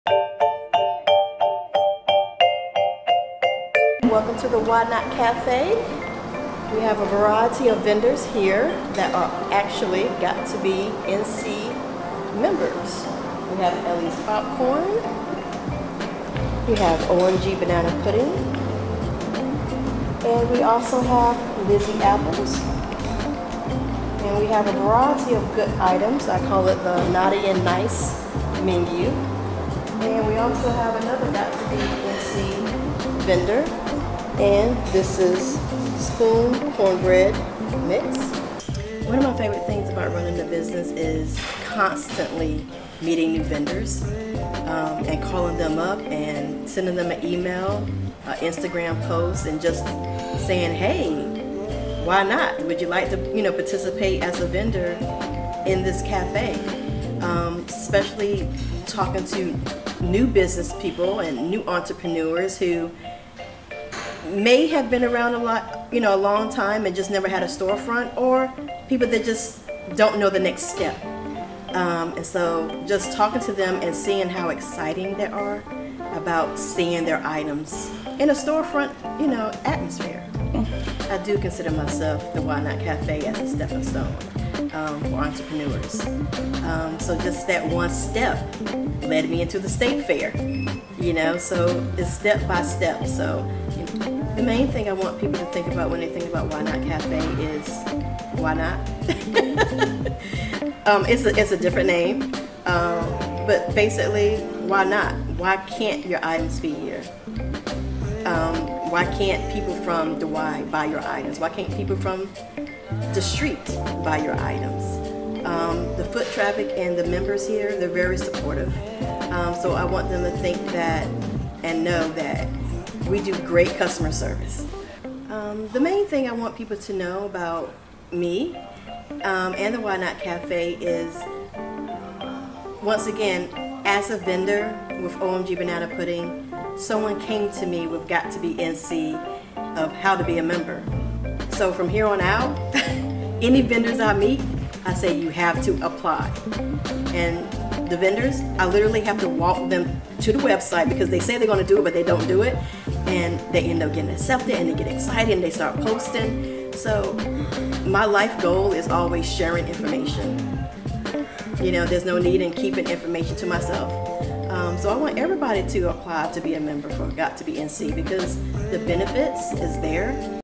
Why Not Cafe
Why Not Café located in the front of the YMCA off Fayetteville Street in Downtown Raleigh offers a variety of local snack and lunch items. Owner and operator, Johnetta prides herself in helping local companies be in a storefront, become Got to Be NC members and encourages them for their next steps. Johnetta also owns, OMG! Banana Pudding which you can find in the Why Not Café.